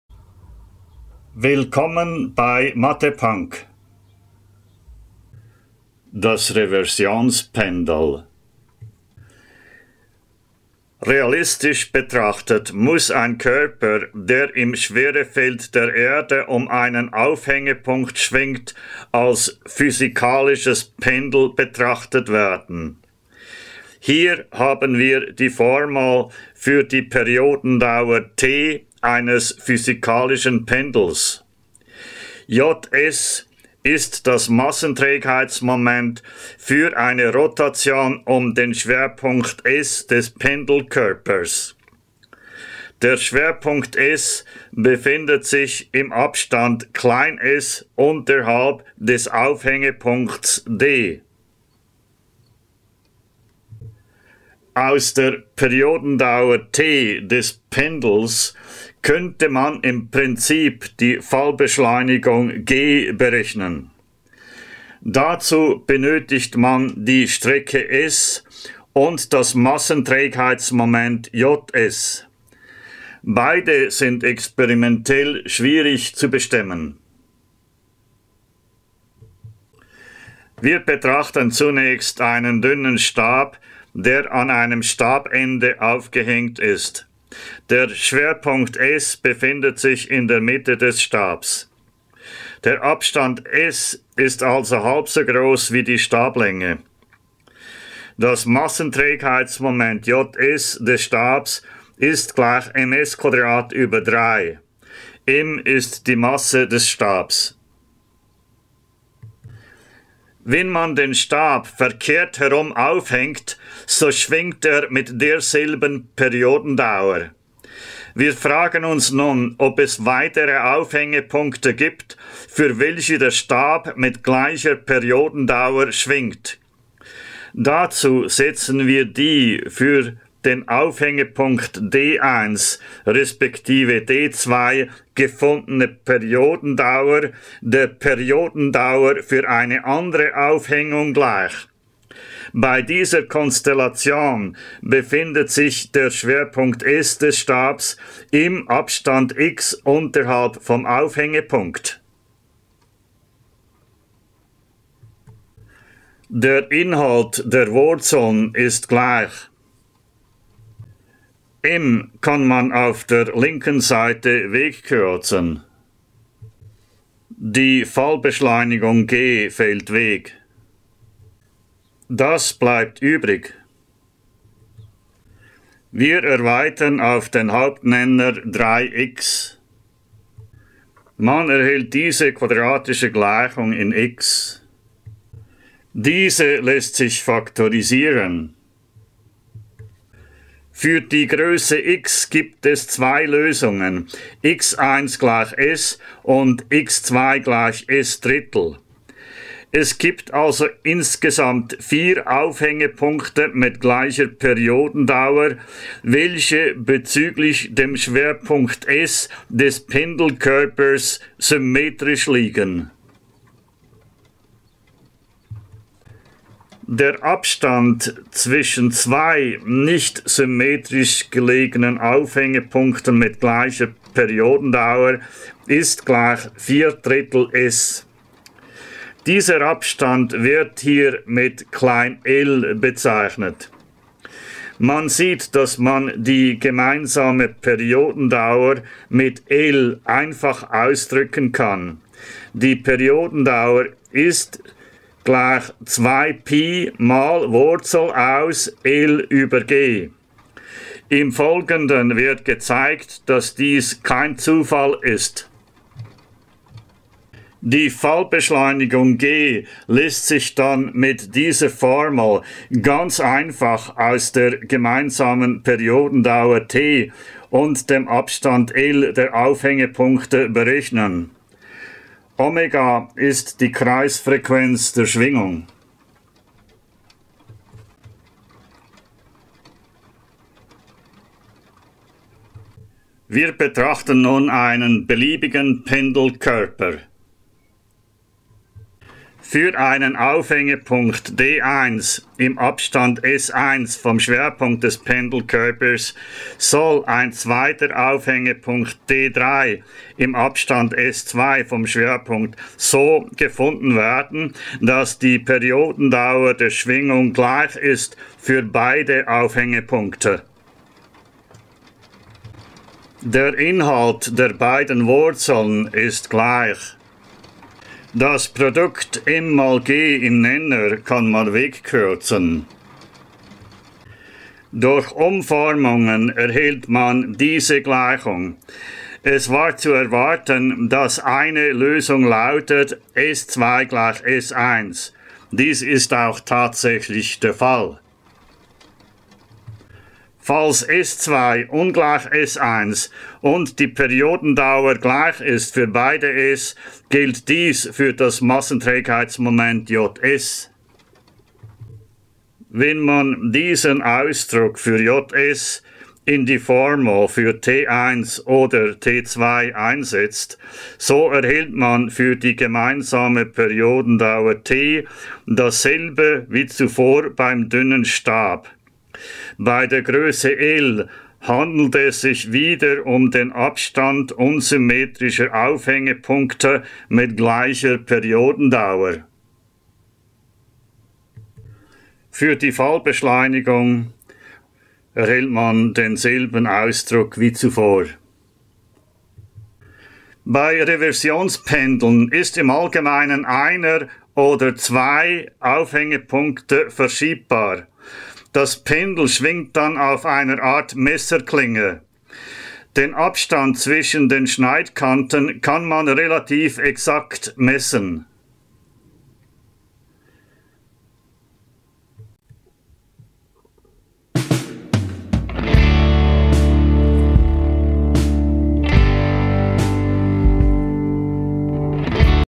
Das Reversionspendel
Ein Reversionspendel ist ein physikalisches Pendel mit zwei Aufhängepunkten mit gleicher Frequenz der Schwingung.  Aus der Frequenz der Schwingung und dem Abstand der beiden Aufhängepunkte mit gleicher Frequenz kann man die Fallbeschleunigung bestimmen.  Das Reversionspendel dient damit als Gravimeter, d.h. als Messinstrument zur Bestimmung der Fallbeschleunigung, resp. Stärke des Gravitationsfelds.  Die Formel zur Berechnung der Fallbeschleunigung wird in diesem Video hergeleitet.